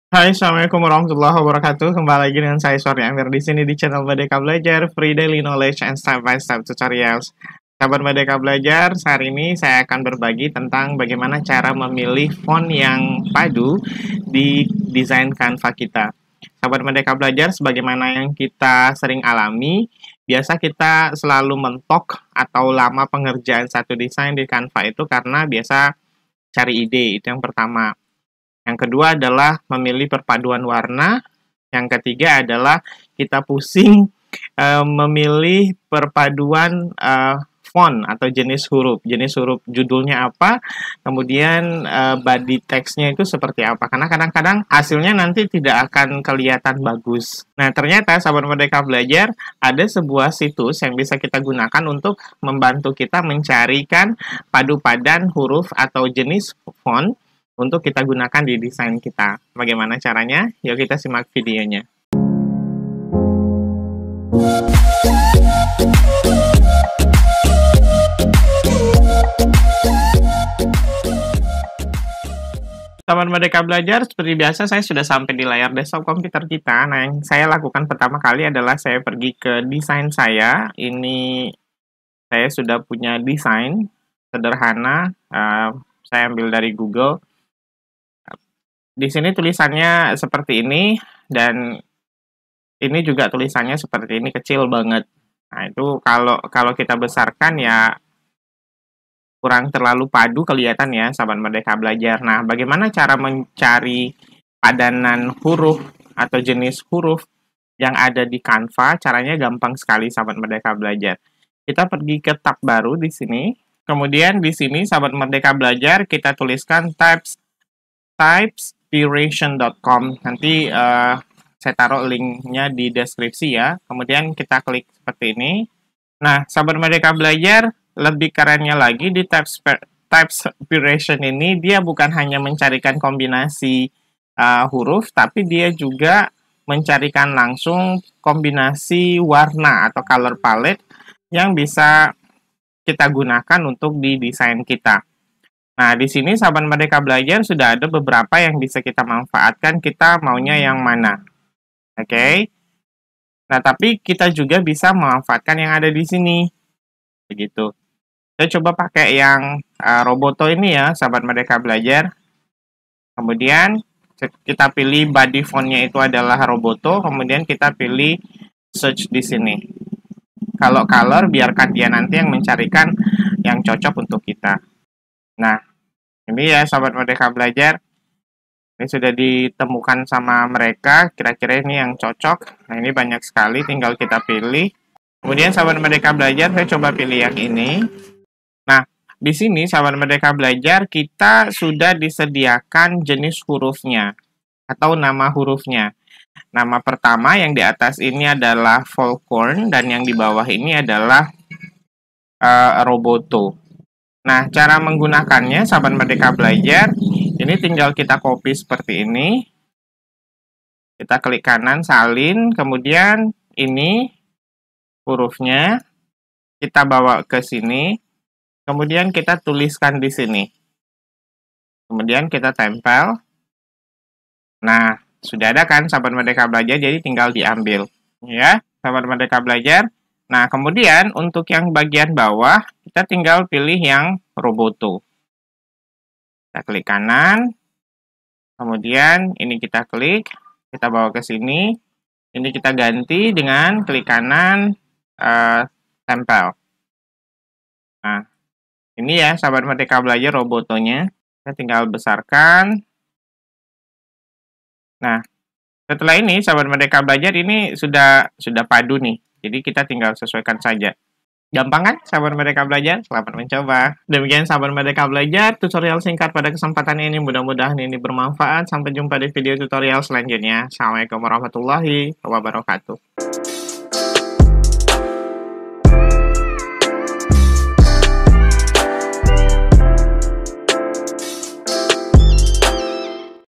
Cara Memilih Jenis Font yang Tepat untuk Desain di Canva
Ada 3 hal yang menurut saya kita terkadang malas untuk mengedit sebuah desain di Canva apa saja itu?
1. Mencari Ide
2. Menemukan Warna yang Padu
3. Menemukan Jenis Huruf/Font yang tepat.

Untuk pemula permasalahan nomor 1 bisa diatasi dengan ATM (Amati Tiru dan Modifikasi) hasil desain orang lain.
Untuk permasalahan nomor 2 sahabat bisa lihat disini

nah untuk permasalahan Nomor 3, yuk sama2 kita belajar cara menemukan jenis huruf/font yang bisa kita gunakan di desain yang kita buat.

semoga menginspirasi.